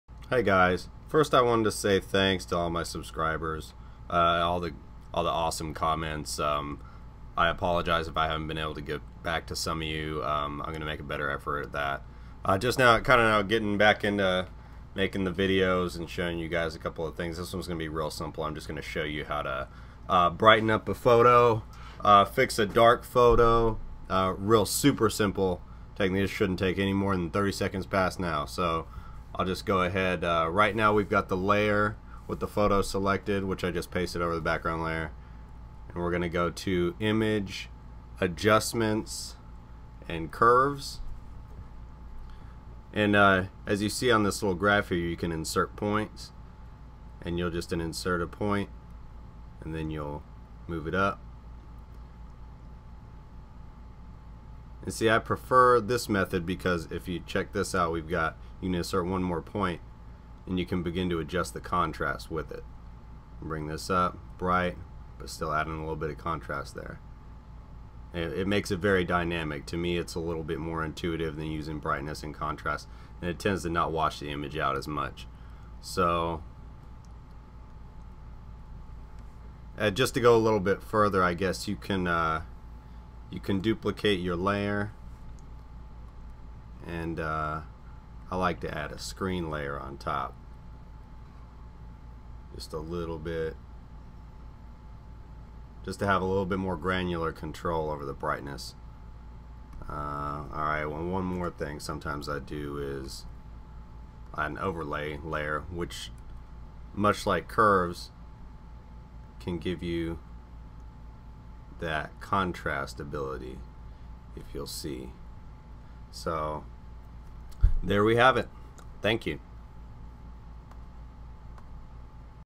How to Fix a Dark Photo in Photoshop
In this Photoshop tutorial, I will show you how to fix a dark photo.  I am using Photoshop CS5, but this technique will work in all versions of Photoshop CS.